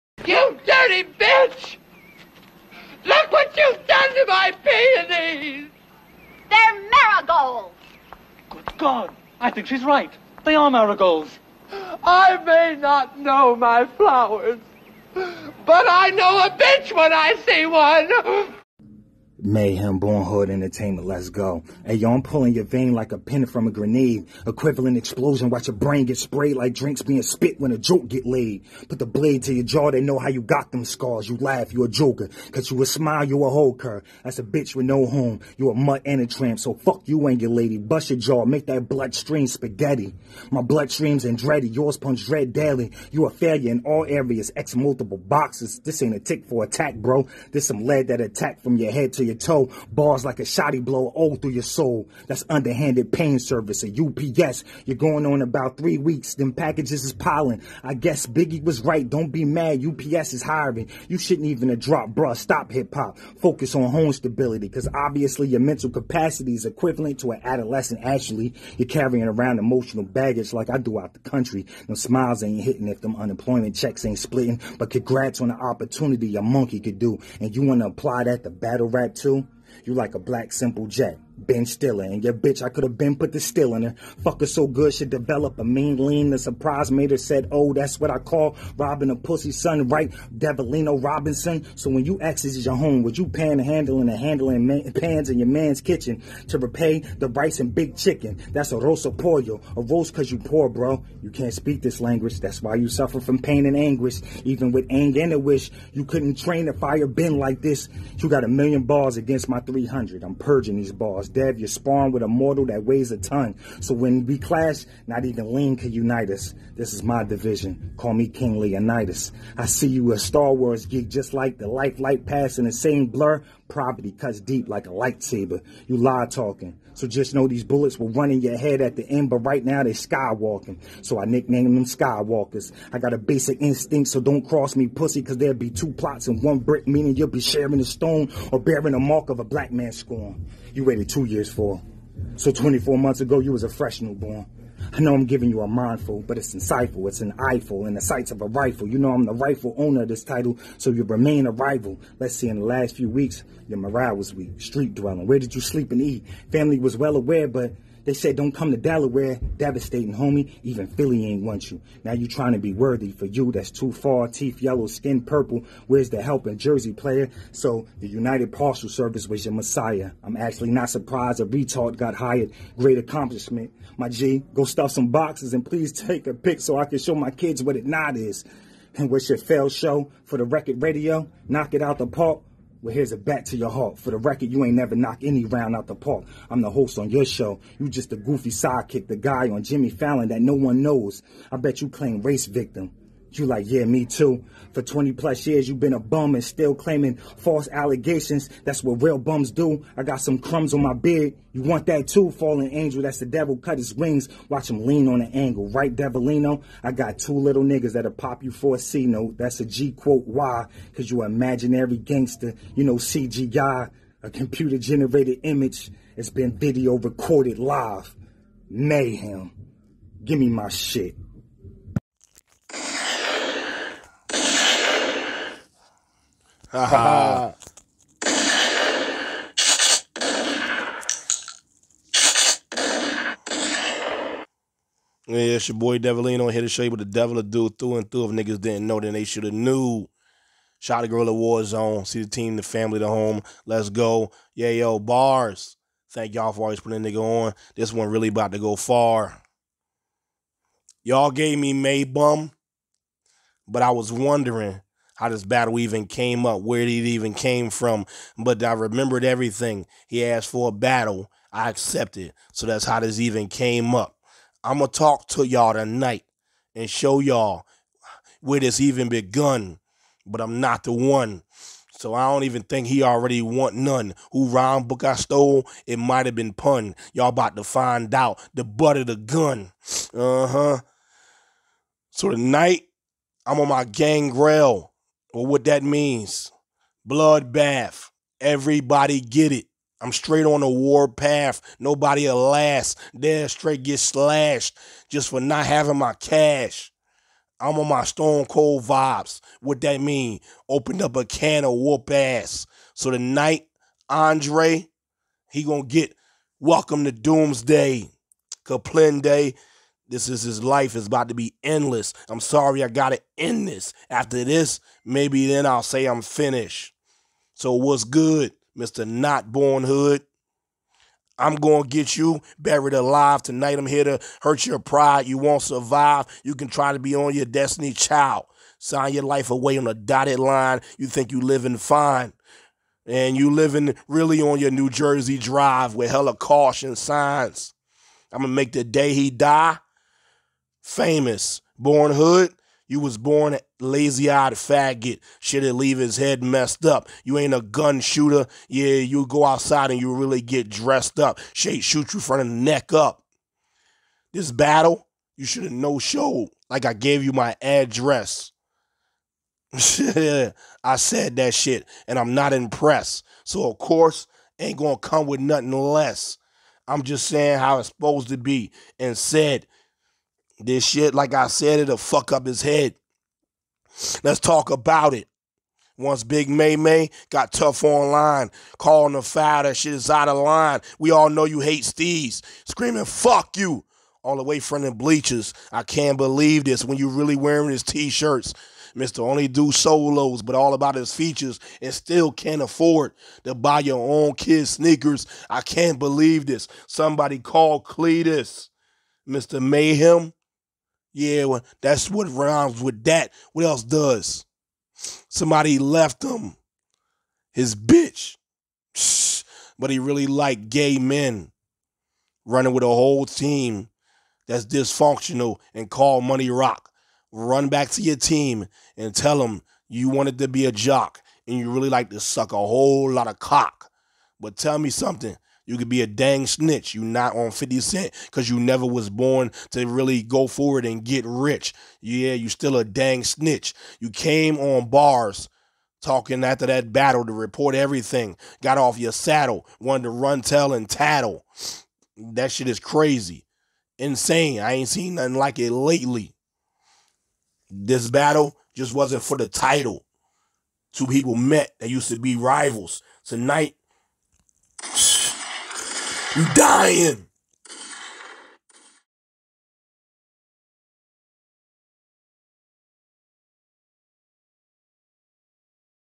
Mayhem vs Devaleno (Audio Battle) (Beef Night 31)
on to our next battle we got an Audio Divsion Battle Between Mayhem vs Devaleno and these guys do not like eachother the beef is real 💯

let's goooo its B.A.R.Z Bitch

who won let us know below